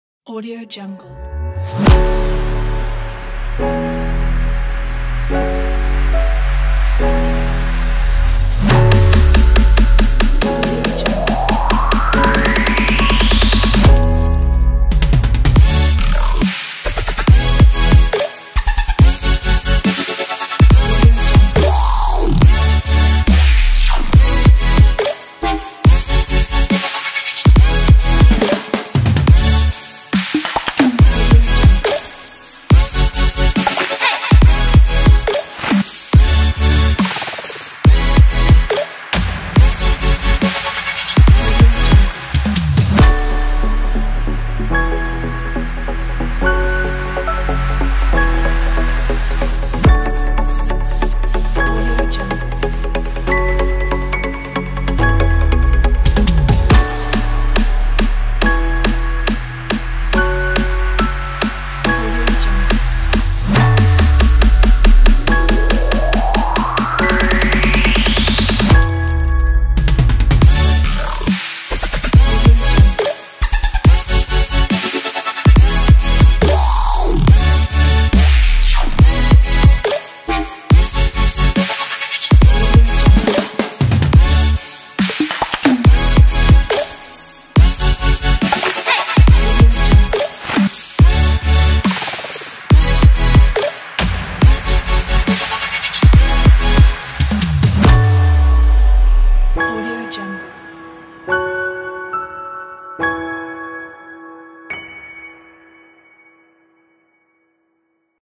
Animated Audio Spectrum Music Visualizer
Music is purchased from here: 1.envato.market/QW4Y6
This project includes 10 Radial Audio Spectrums, 4 Horizontal Spectrums, 1 Animated Avatar. All the animations are driven by your music track. Easy to use. Add your audio, background image, set your colors and you are done. Use this for Youtube Channels, Audio React, Audio Visualization, Waveform and Music Spectrum Equalizer.